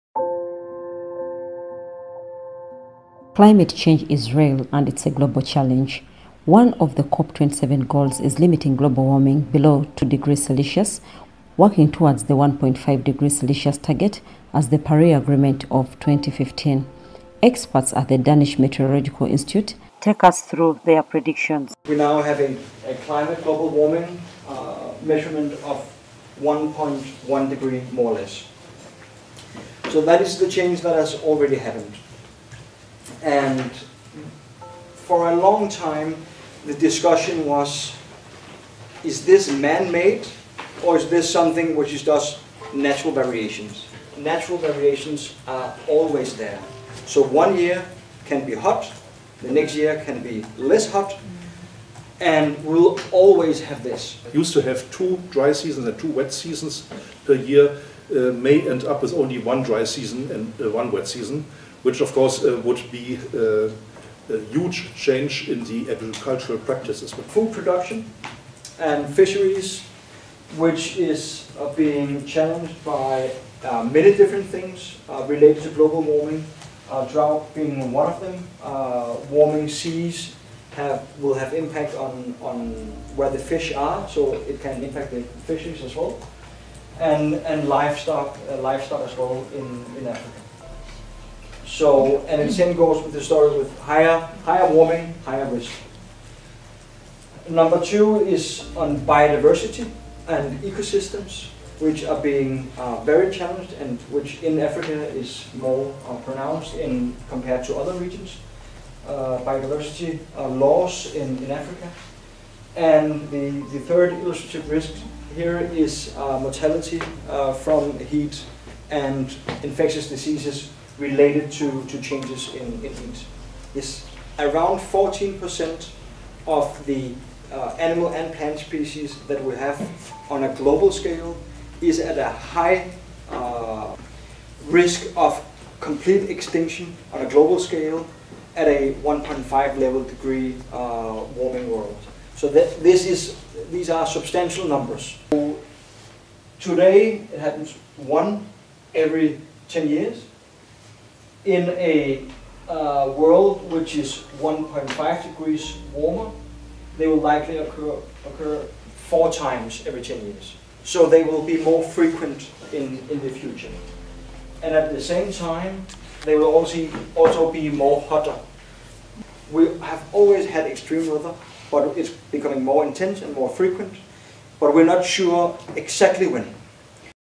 Climate Experts from Denmark speak out.
Experts predict what may happen in the coming 10 years if Global warming persists